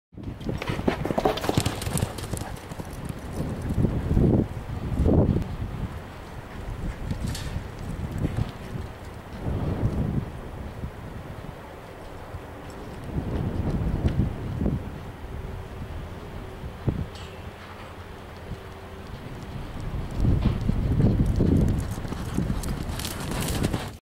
Phoenix Sundre Time Only
Whos Bad 4 year old mare second time away from home.